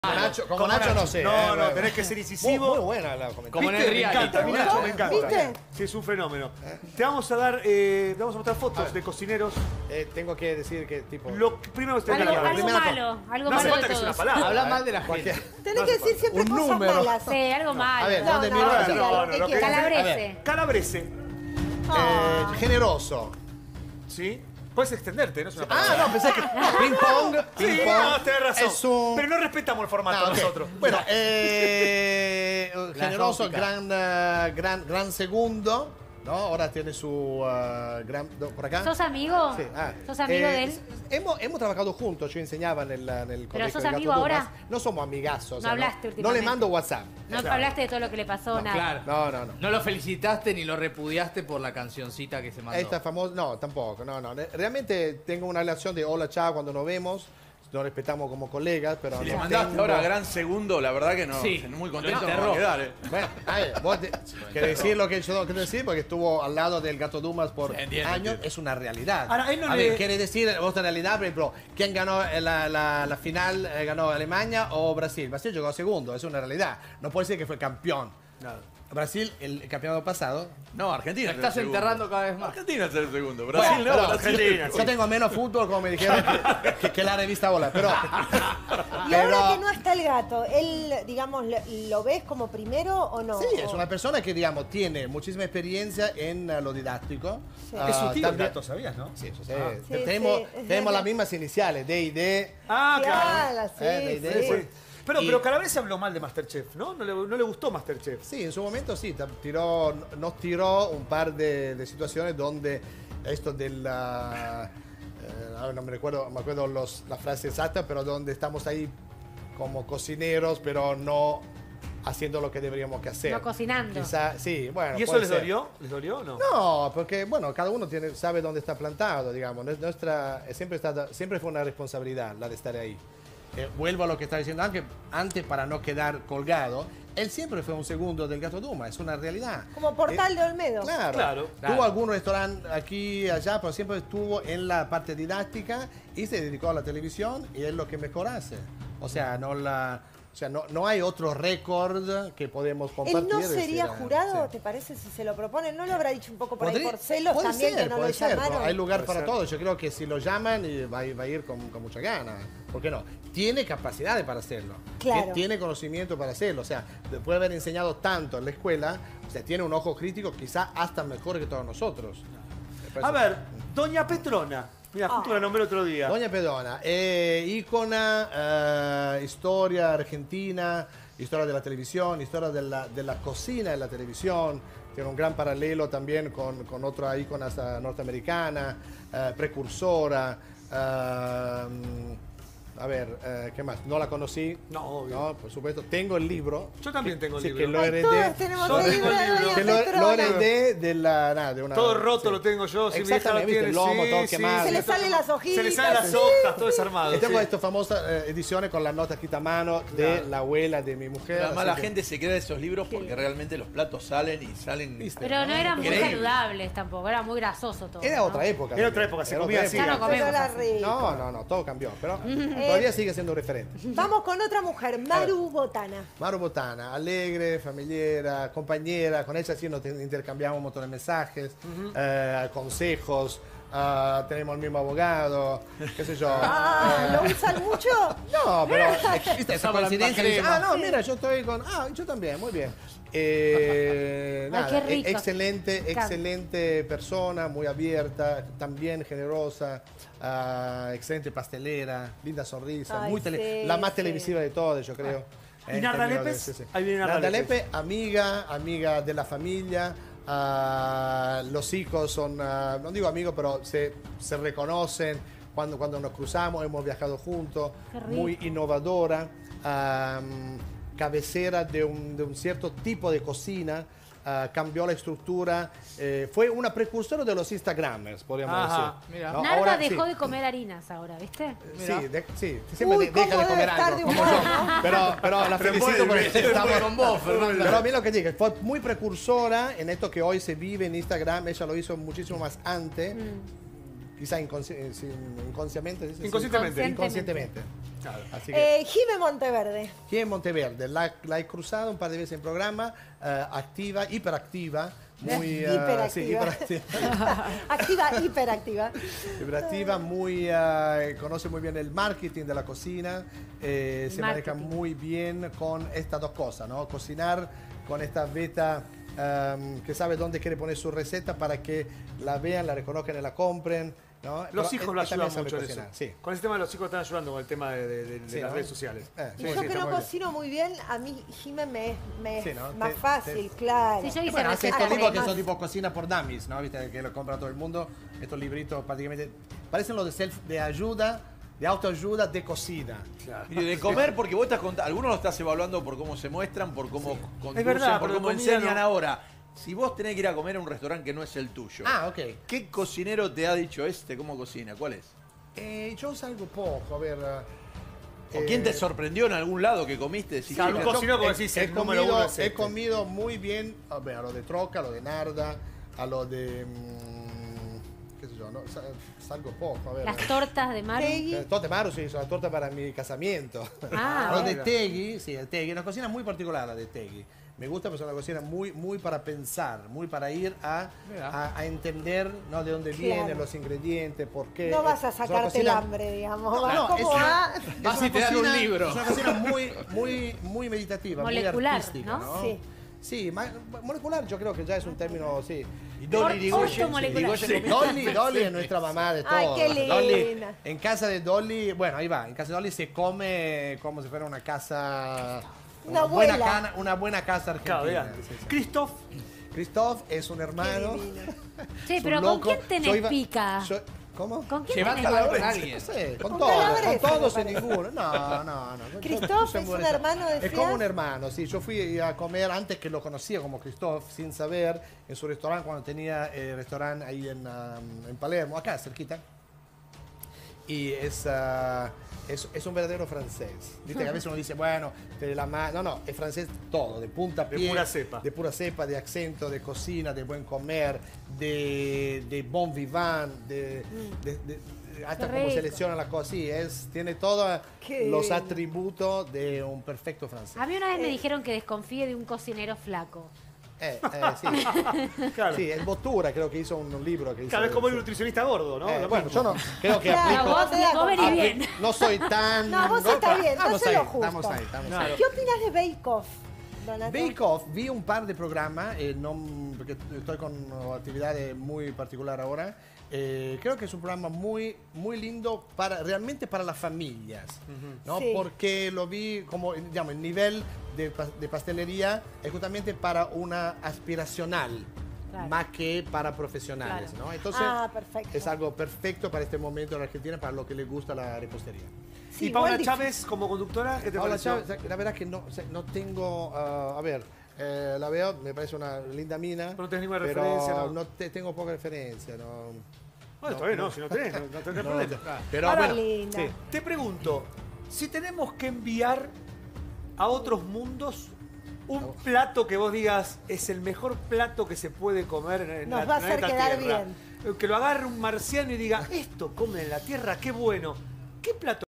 Donato De Santis habló sobre los otros cocineros
De Santis: "Calabrese es un gran segundo". El cocinero de las celebridades opinó sobre sus otros colegas, con muchos asegura que tiene relación de hola y chao. Mencionó a Doña Petrona como uno de los iconos de la cocina.